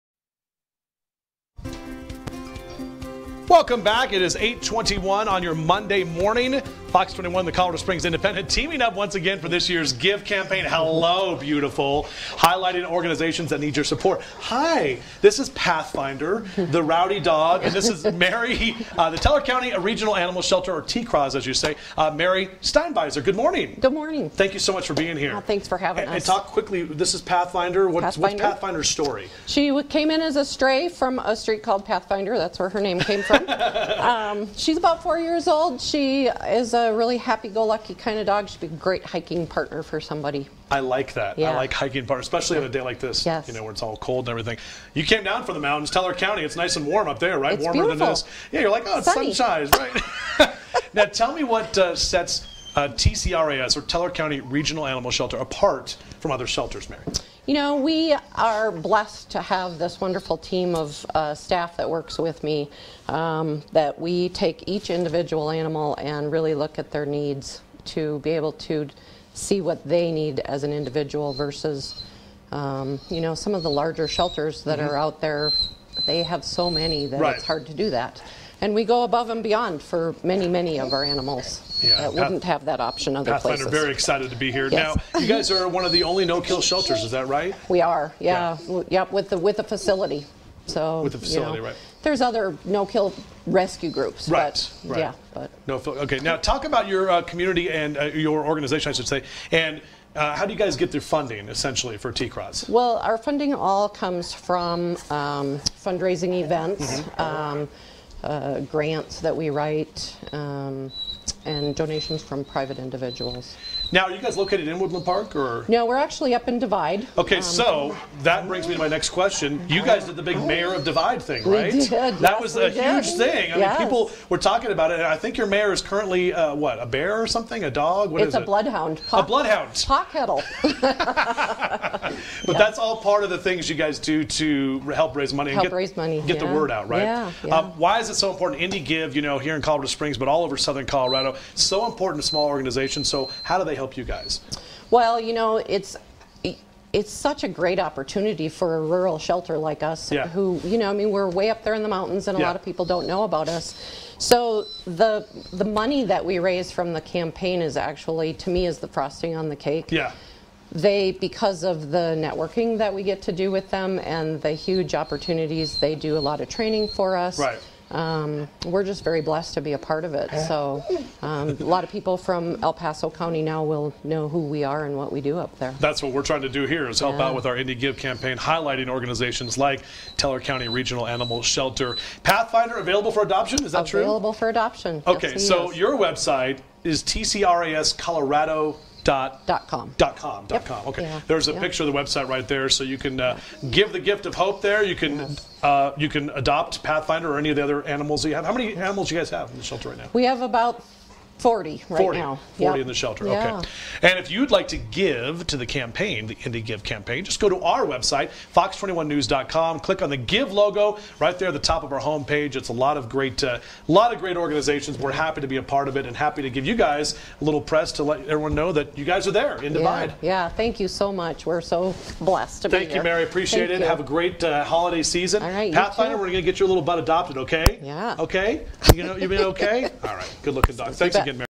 Give!: Teller County Regional Animal Shelter
Mary Steinbeiser tells us about Teller County Regional Animal Shelter.